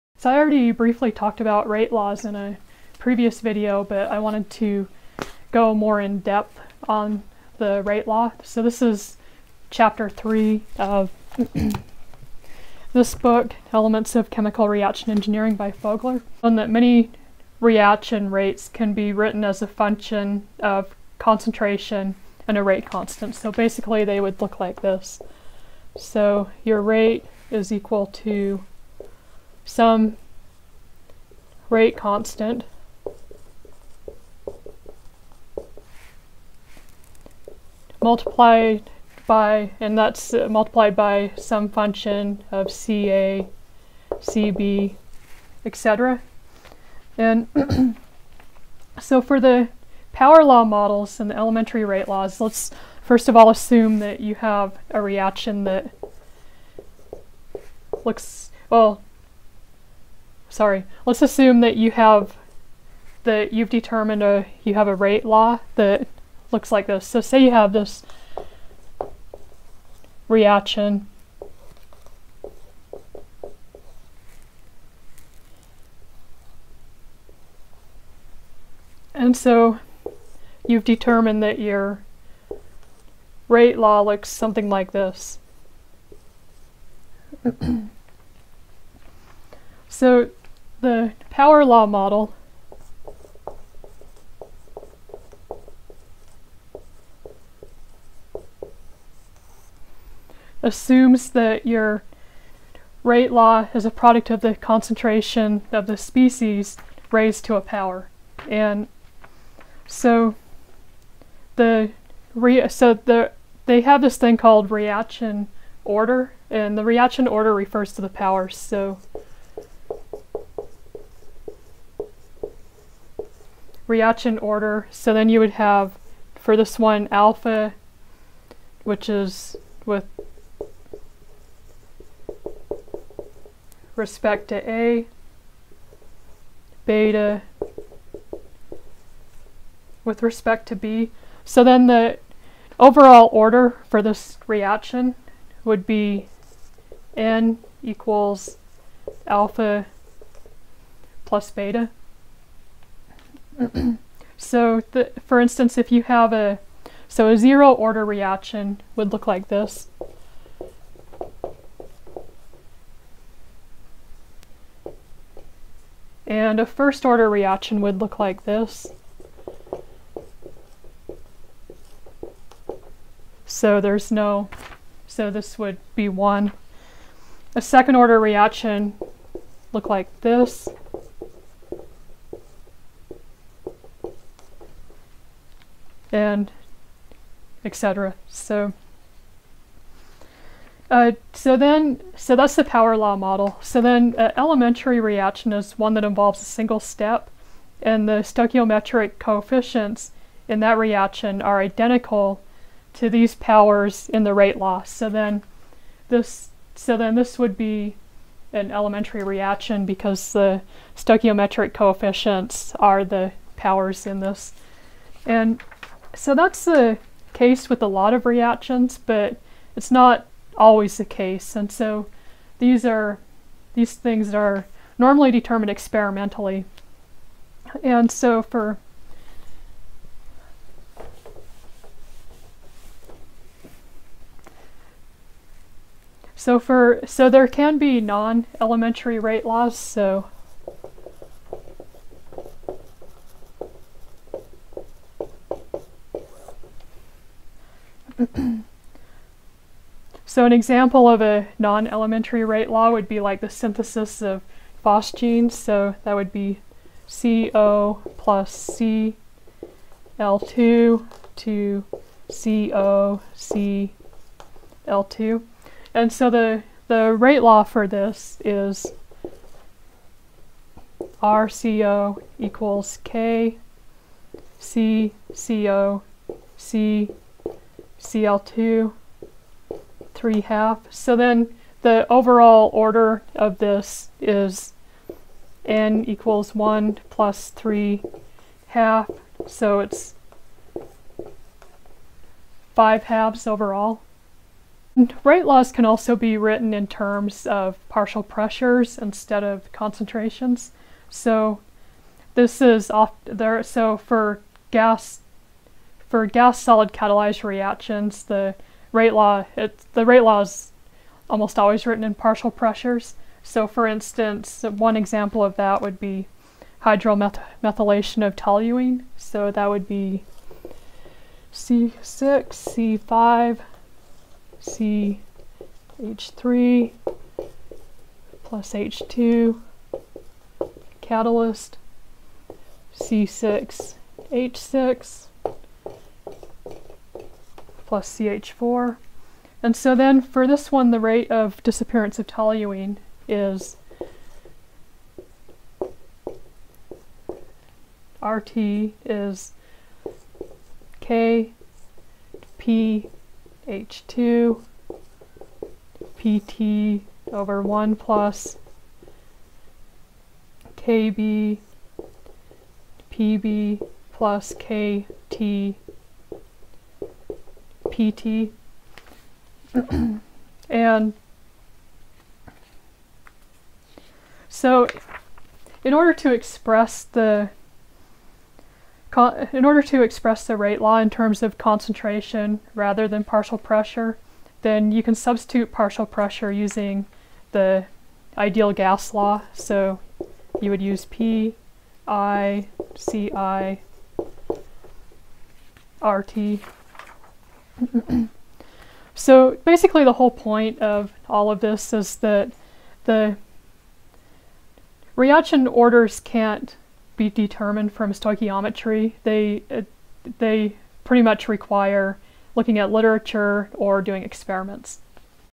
10) Reaction Engineering, Elementary, non-Elementary Rate Laws
Power law model, elementary rate law, non-elementary and rate laws with partial pressure.